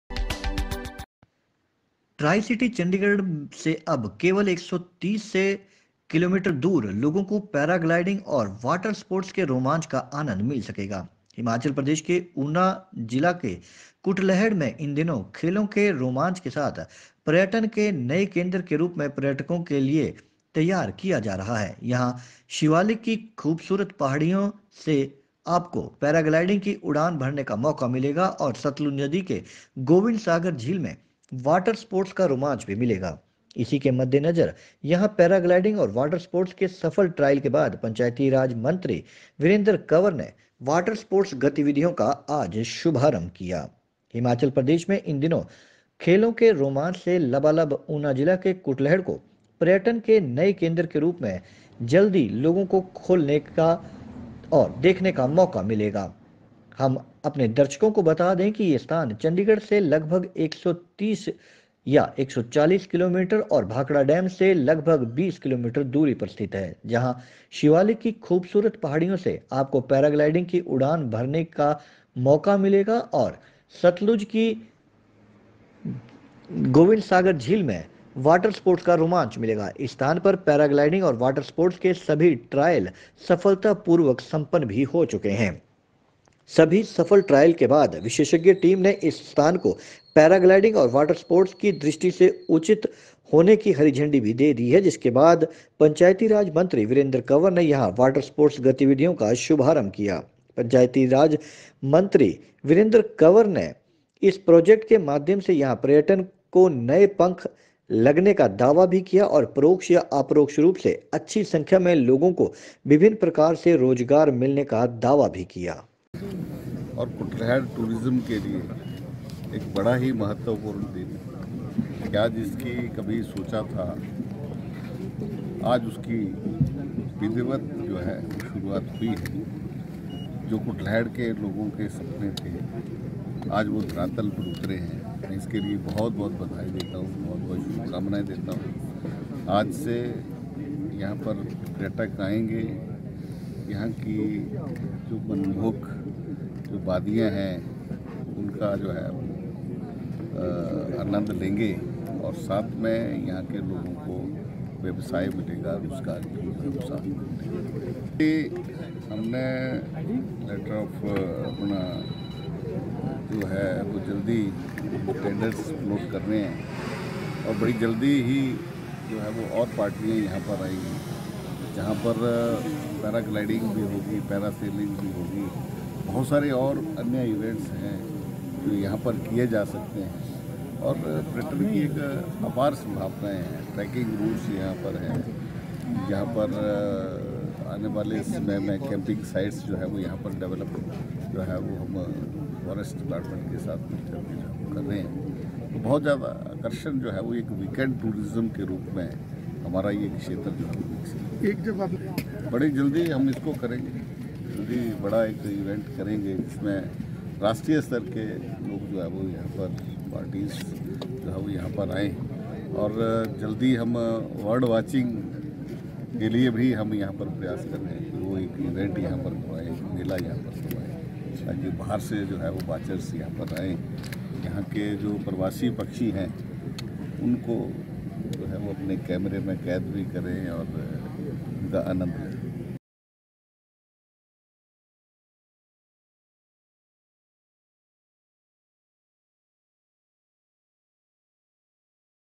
Minister Virendra Kanwar launched the water sports & paragliding activities at UNA
ट्राइसिटी चंडीगढ़ से अब केवल 130 से 140 किलोमीटर दूर लोगों को पैराग्लाइडिंग और वाटर स्पोर्ट्स के रोमांच का आनंद मिल सकेगा । हिमाचल प्रदेश के ऊना जिले का कुटलैहड़ इन दोनों खेलों के रोमांच के साथ पर्यटन के नए केंद्र के रूप में पर्यटकों के लिए लगभग तैयार है ।
यहाँ शिवालिक की खूबसूरत पहाड़ियों से आपको पैराग्लाइडिंग की उड़ान भरने को मिलेगी और सतलुज नदी की गोविंद सागर झील में वाटर स्पोर्ट्स का रोमांच मिलेगा । इसी के मद्देनजर यहां पैराग्लाइडिंग और वॉटर स्पोर्ट्स के सफल ट्रायल के बाद पंचायती राज मंत्री वीरेंद्र कँवर ने वाटर स्पोर्ट गतिविधियों का शुभारंभ किया ।